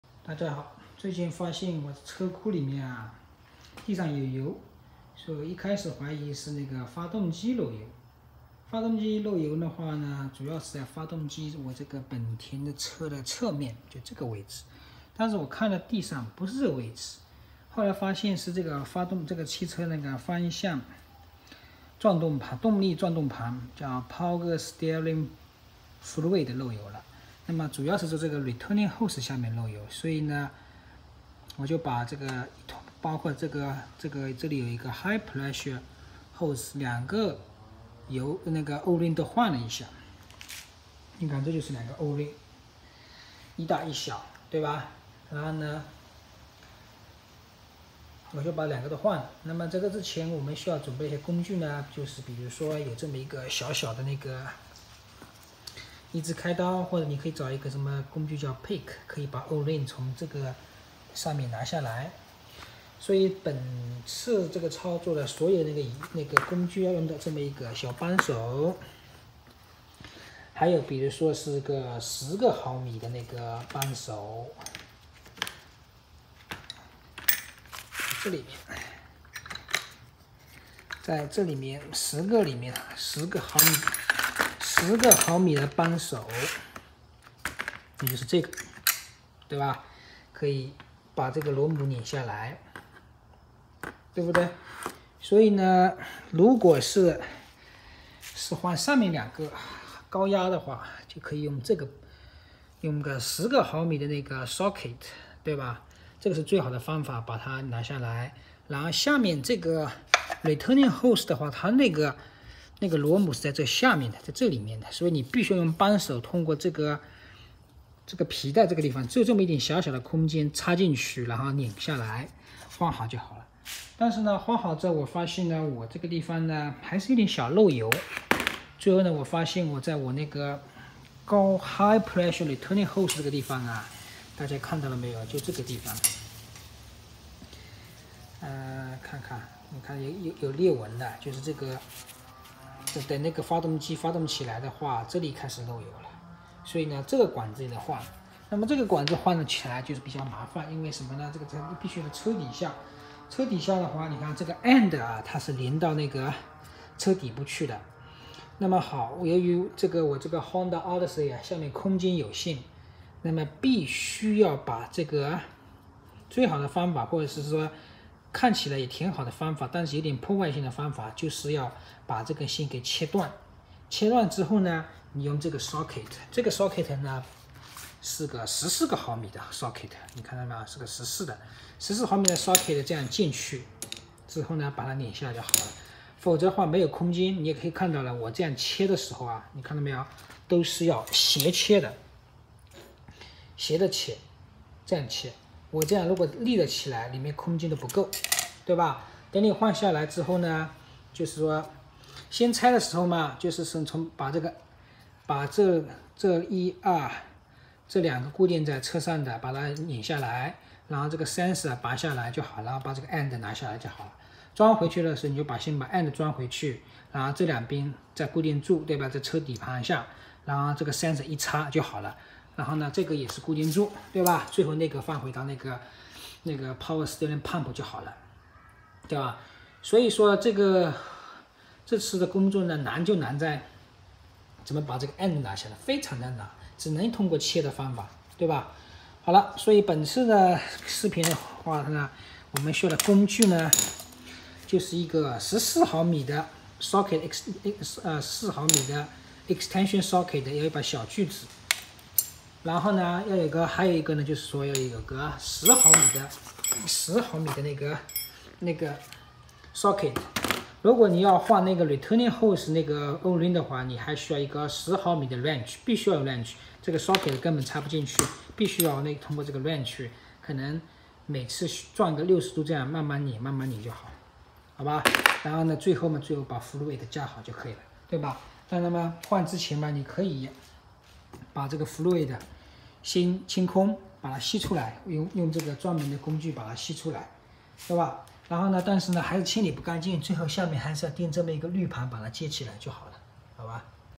1. DIY Replace 2007 Honda Odyssey Power Steering High-Pressure Hose 本田奥德赛替换动力方向盘高压软管
2007年本田奥德赛换动力方向盘高压软管